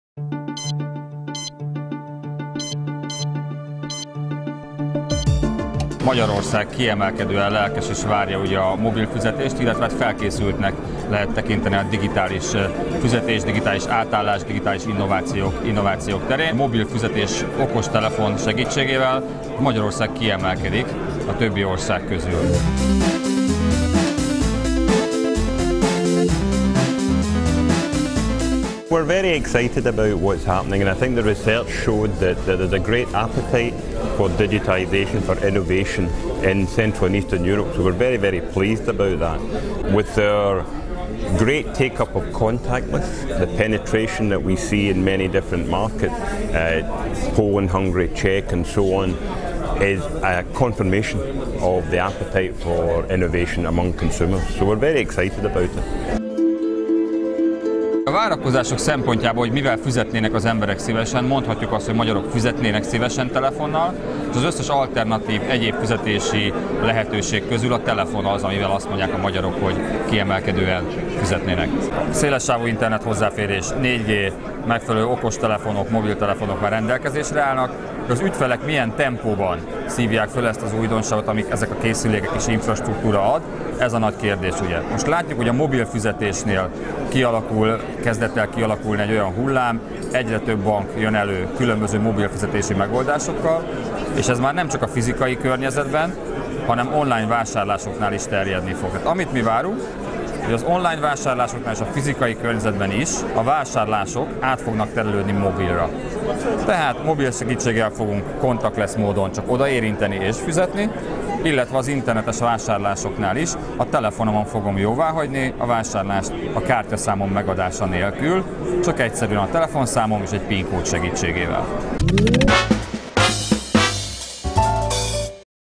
Leginkább mobiltelefonra cserélnénk le a bankkártyánkat
Elérkezett az az idő, amikor még a bankkártyánkat is a "beszippantja" a mobiltelefonunk. Legalább is erre következtethetünk a Mastercard kutatásából, ugyanis leginkább ez a technológia foglalkoztatja a magyar fogyasztókat. A fizikai és az internetes vásárlás is mobilra tart!